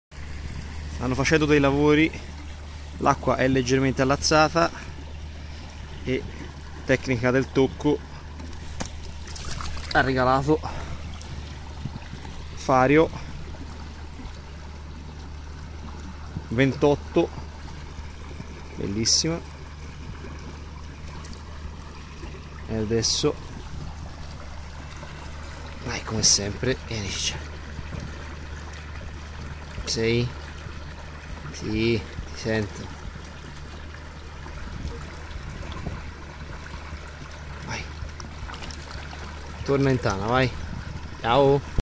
Pesca al tocco nei fossi di Toscana
Insistendo quasi 5 minuti senza aver sentito una tocca,alla fine è uscita lei,come per magia.